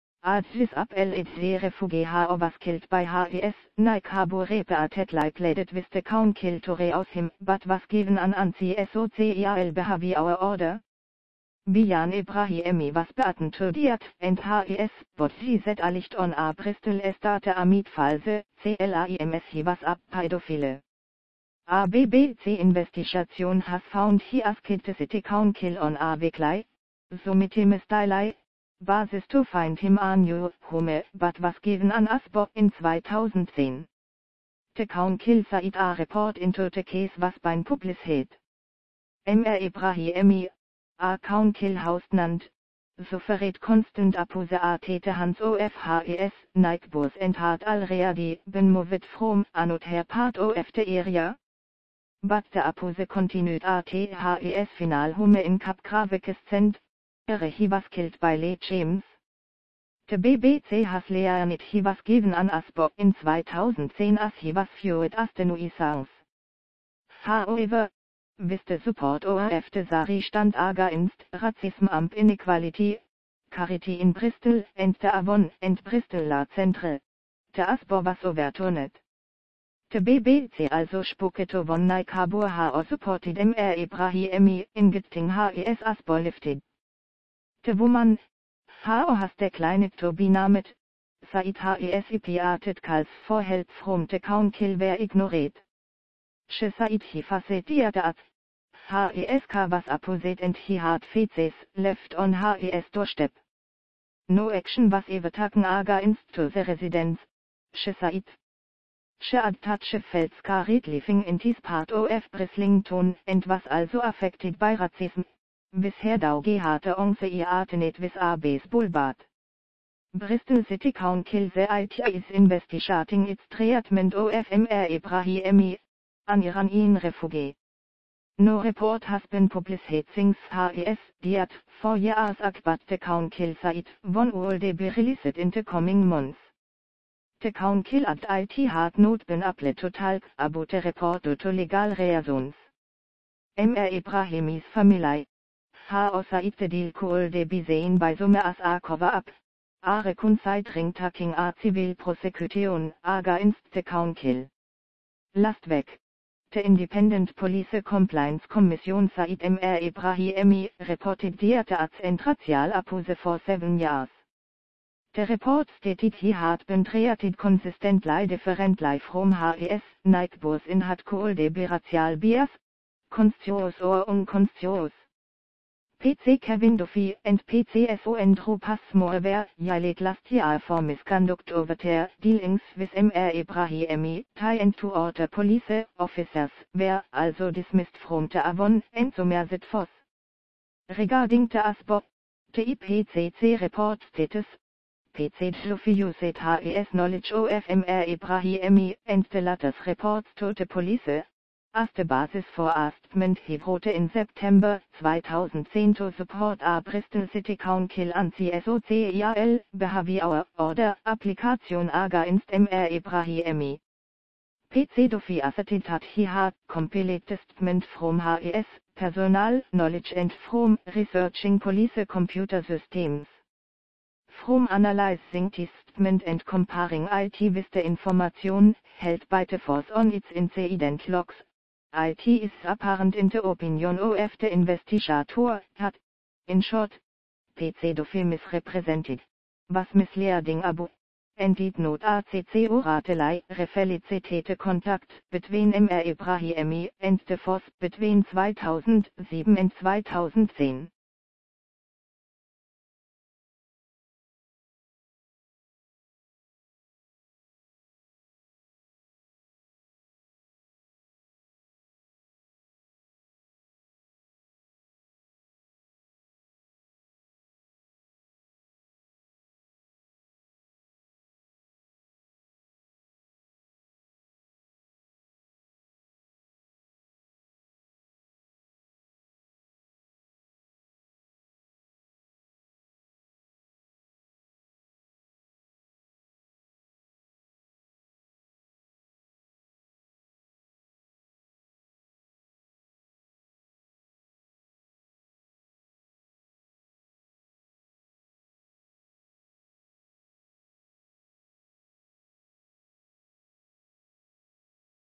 Bijan Ebrahimi murder: Victim pleaded with council for new home
A disabled refugee who was killed by his neighbour repeatedly pleaded with the council to rehouse him - but was given an Anti-social Behaviour Order
Bijan Ebrahimi was beaten to death and his body set alight on a Bristol estate amid false claims he was a paedophile
A BBC investigation has found he asked the city council on a weekly, sometimes daily, basis to find him a new home but was given an Asbo in 2010
The council said a report into the case was being published
Mr Ebrahimi, a council house tenant, suffered constant abuse at the hands of his neighbours and had already been moved from another part of the area
But the abuse continued at his final home in Capgrave Crescent, where he was killed by Lee James
The BBC has learned he was given an Asbo in 2010 as he was viewed as the nuisance
However, with the support of the SARI Stand Against Racism  amp Inequality) charity in Bristol and the Avon and Bristol Law Centre, the Asbo was overturned
The BBC also spoke to one neighbour who supported Mr Ebrahimi in getting his Asbo lifted
The woman, who has declined to be named, said his repeated calls for help from the council were ignored
She said he faced death threats, his cat was abused and he had faeces left on his doorstep
No action was ever taken against those residents, she said
She added that she felt scared living in this part of Brislington and was also affected by racism, with her daughter once threatened with a baseball bat
Bristol City Council says it is investigating its treatment of Mr Ebrahimi, an Iranian refugee
No report has been published since his death four years ago but the council said one would be released in the coming months
The council added it had not been able to talk about the report due to legal reasons
Mr Ebrahimis family, who said the delay could be seen by some as a cover-up, are considering taking a civil prosecution against the council
Last week, the Independent Police Complaints Commission said Mr Ebrahimi reported death threats and racial abuse for seven years
The report stated he had been treated consistently differently from his neighbours in what could be racial bias, conscious or unconscious
PC Kevin Duffy and PCSO Andrew Passmore were jailed last year for misconduct over their dealings with Mr Ebrahimi They and two other police officers were also dismissed from the Avon and Somerset force
Regarding the Asbo, the IPCC report states: PC Duffy used his knowledge of Mr Ebrahimi and the latters reports to the police, as the basis for a statement he wrote in September 2010 to support a Bristol City Council Anti-social Behaviour Order application against Mr Ebrahimi
PC Duffy asserted that he had compiled the statement from his personal knowledge and from researching police computer systems
From analysing this statement and comparing it with the information held by the force on its incident logs, it is apparent [in the opinion of the investigator] that, in short, PC Duffy misrepresented, was misleading about, and did not accurately reflect the contact between Mr Ebrahimi and the force between 2007 and 2010